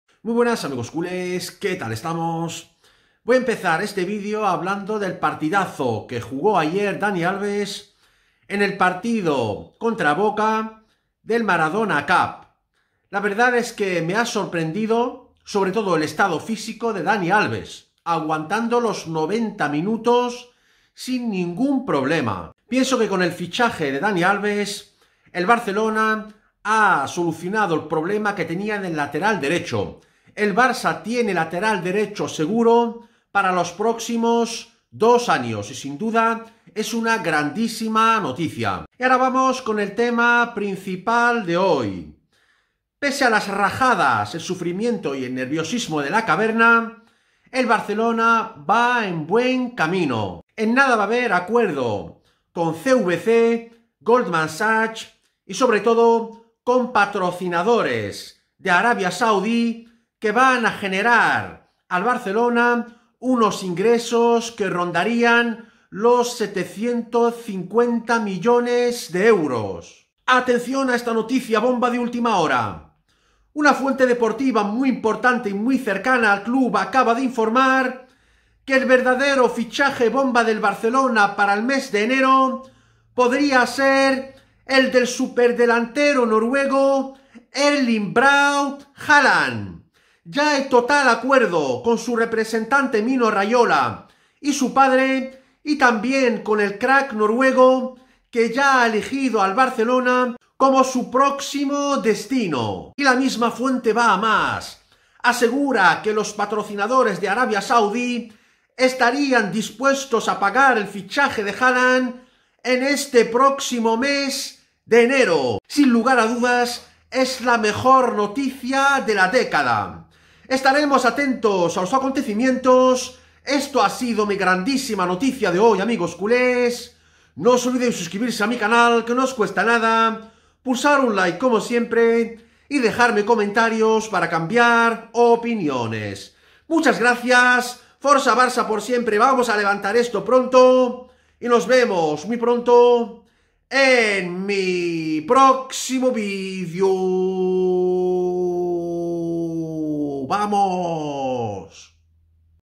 BOMBA MUNDIAL: ¡FICHAJE DE HAALAND POR EL BARCELONA EN ENERO!
Confirman el fichaje de Haaland por el Barcelona en Enero